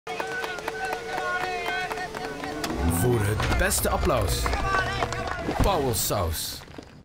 Pauwels Suzen sponsort Brico Cross 2017
Pauwels Sauzen is de trotse sponsor van de Brico Cross 2017! Ook het team van Pauwels Sauzen Vastgoedservice zal te zien zijn op VTM!